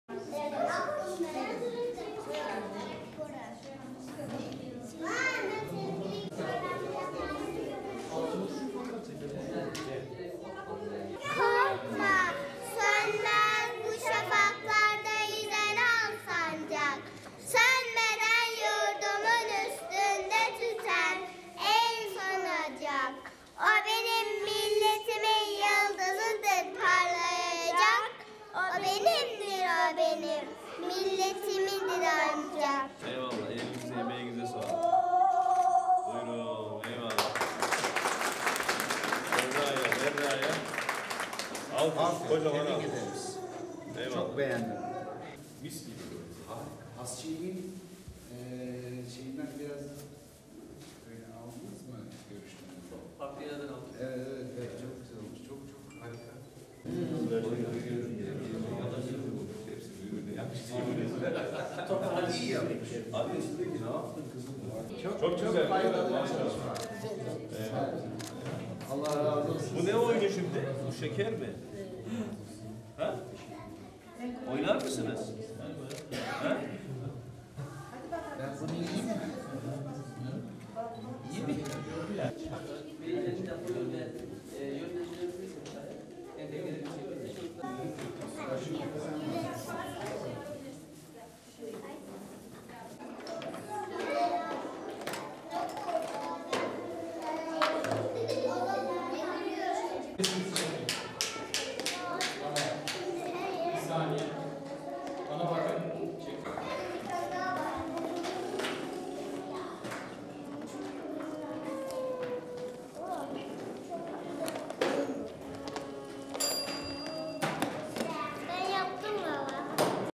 MİNİKLERE AKIL OYUNLARI
Hacılar Kudsiye Şaşoğlu Kuran Kursu Akıl Oyunları ve Lego Atölyesi düzenlenen törenle açıldı.

4-6 yaş grubu öğrencilerin kullanımına sunulan Akıl Oyunları ve Lego Atölyesi açılış törenine Hacılar Kaymakamı Haluk Sezen, Belediye Başkanı Bilal Özdoğan, İlçe Jandarma Komutanı Muhittin Doğan, Hayırsever Kudsiye Şaşoğlu, kurum amirleri, öğrenciler, öğretmenler ve veliler katıldı.